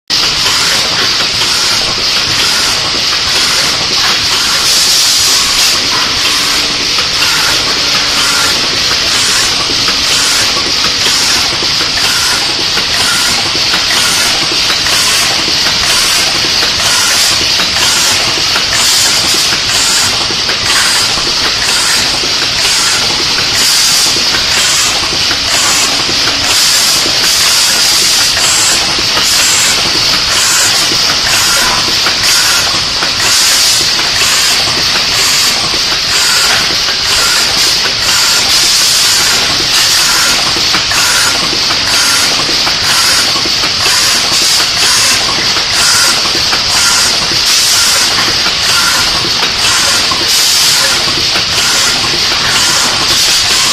Auto Foil Sealing and Capping Machine
Function: for sticking aluminum foil on tube orifice and scrwing cap on tube head.